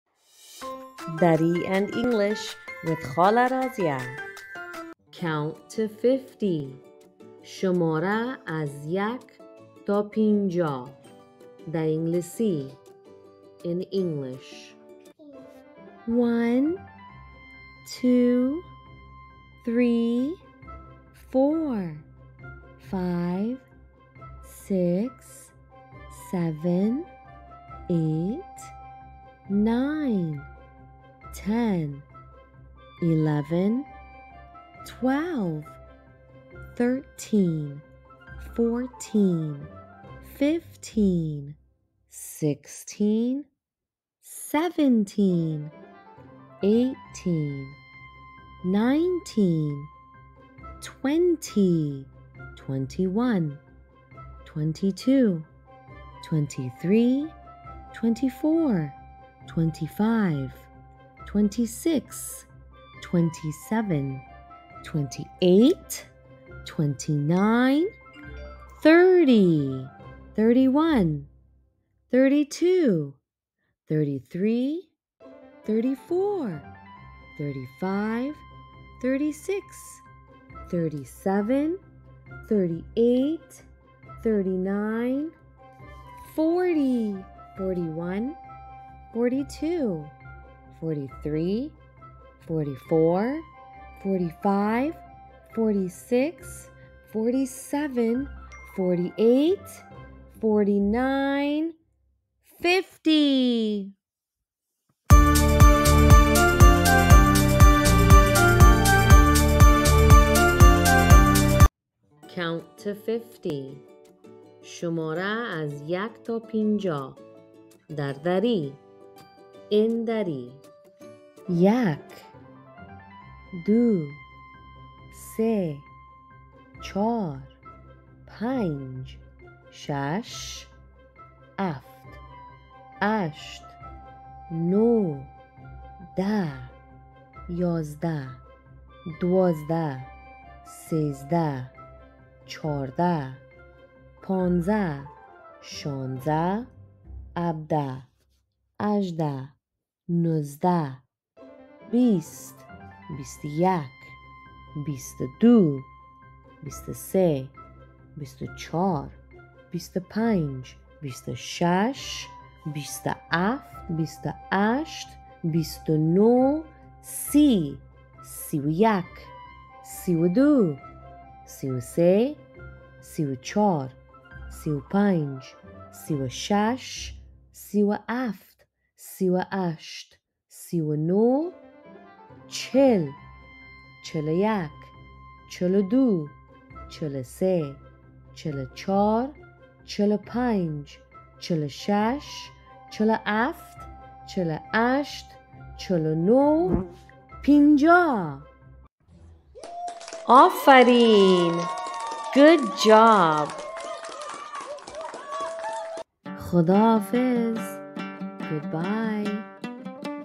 Count to 50 | Bilingual Education | Dari and English
Learning to count to 50 develops foundational math skills, enhances cognitive functions like logic and memory, improves problem-solving abilities, and prepares children for everyday tasks involving numbers. This skill is crucial for a child's academic success and builds confidence in their ability to handle mathematical concepts as they grow.